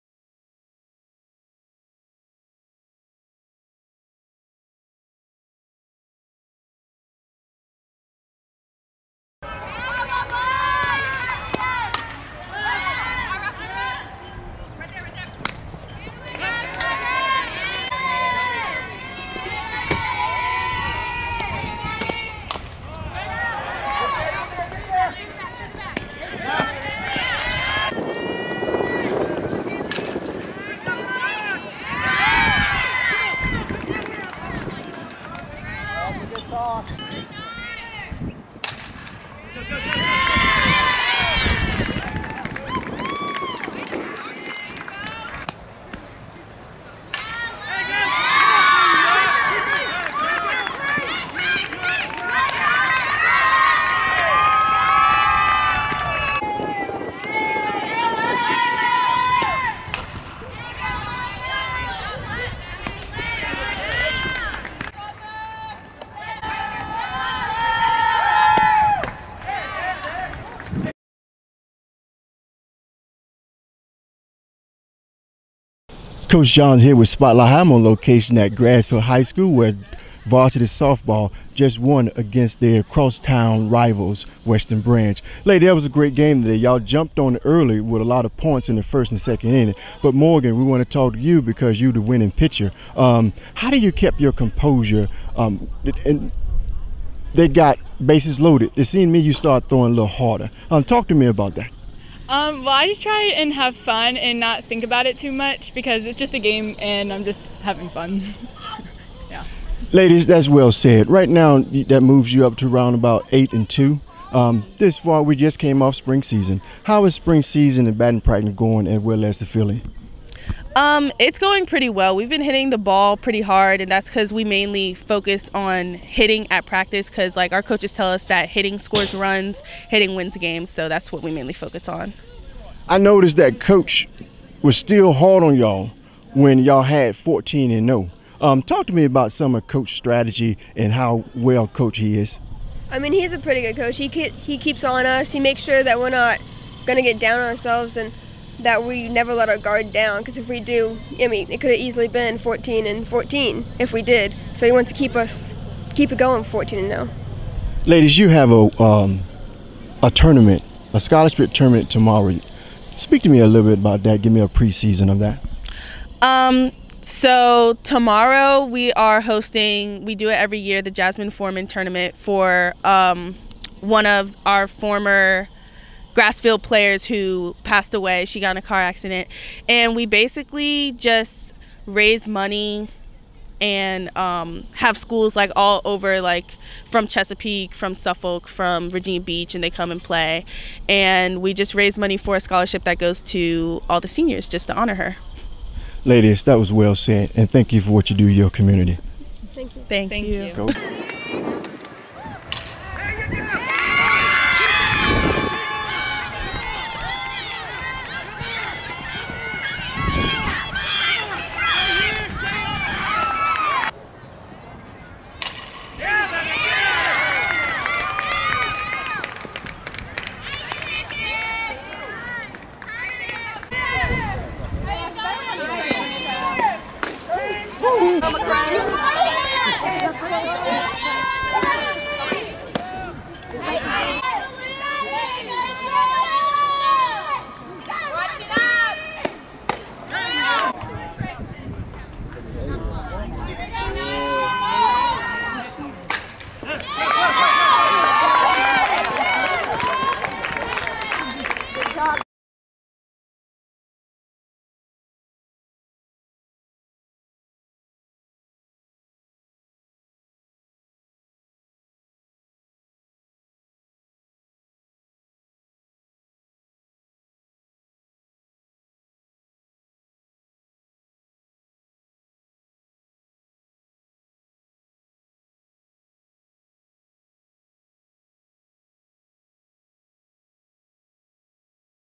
Coach Johnson speaks with the Grassfield H.S. ladies Grizzlies as they reflect on a big win.
Coach Johnson speaks with the Grassfield H.S. ladies Grizzlies as they reflect on a big win with cross town rivals Western Branch H.S.
Nzingha Florence #-4
Morgan Murphy #-9
Sydnie Matkins #-8